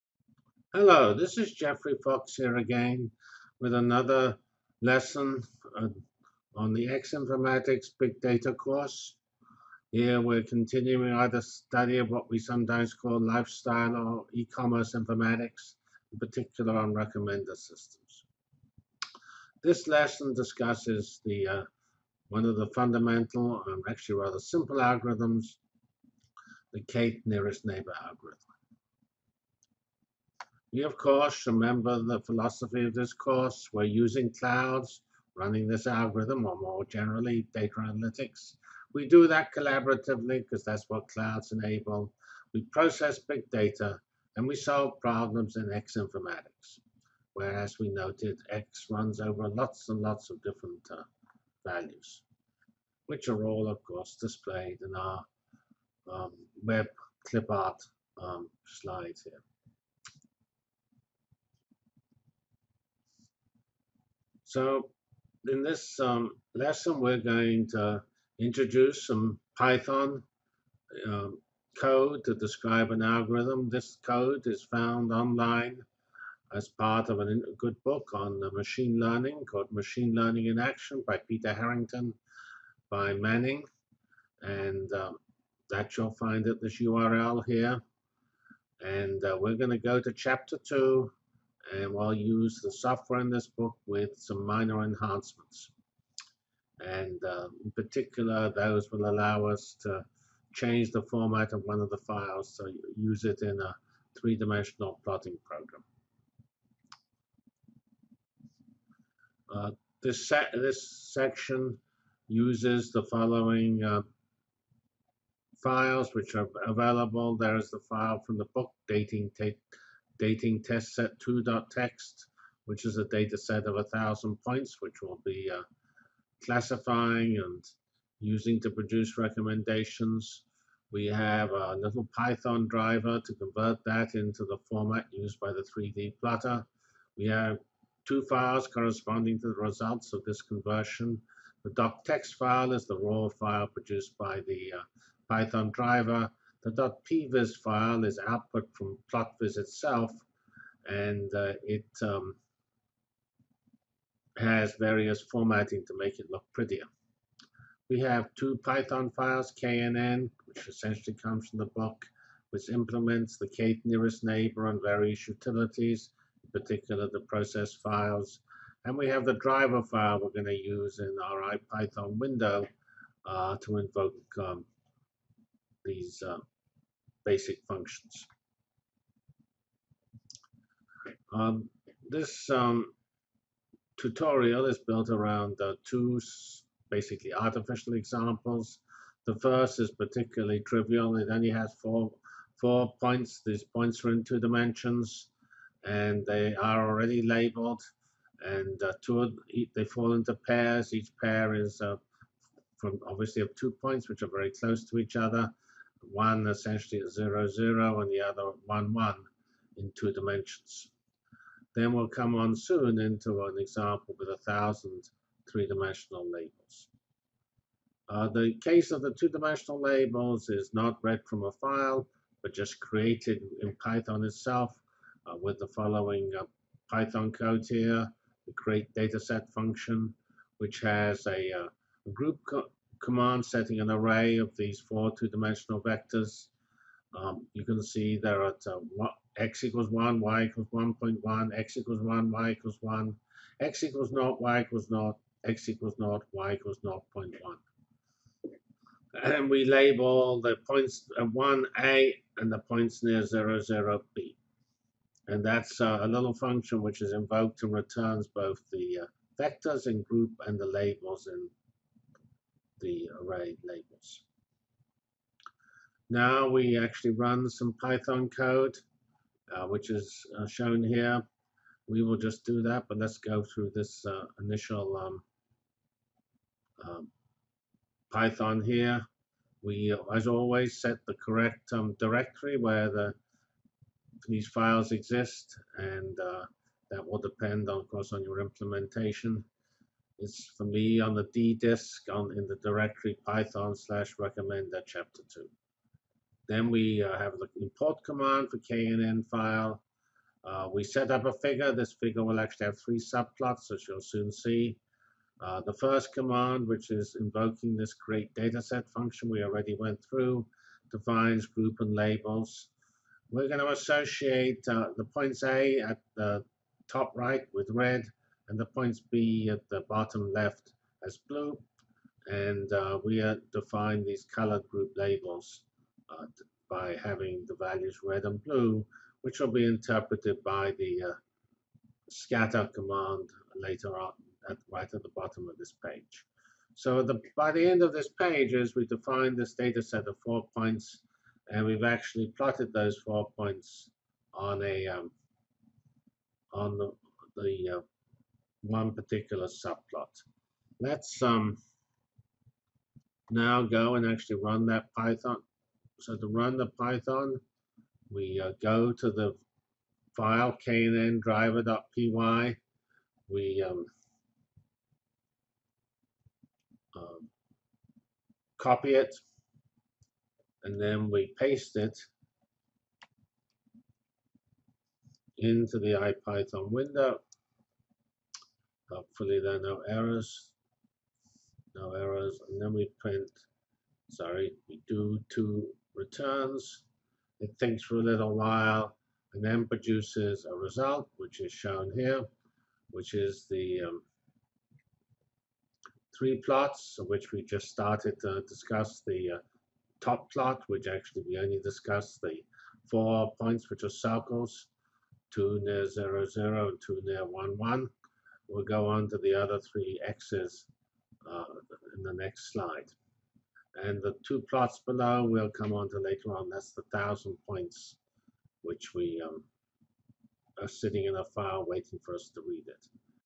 IU X-Informatics Unit 15 : Recommender System IV 1: Python k'th Nearest Neighbor Algorithms I
Lesson Overview:
This lesson considers the Python k Nearest Neighbor code found on the web associated with a book by Harrington on Machine Learning. There are two data sets. First we consider a set of 4 2D vectors divided into two categories (clusters) and use k=3 Nearest Neighbor algorithm to classify 3 test points. Second we consider a 3D dataset that has already been classified and show how to normalize. In this lesson we just use Matplotlib to give 2D plots

and download course material, see information on badges and more. It's all free and only takes you a few seconds.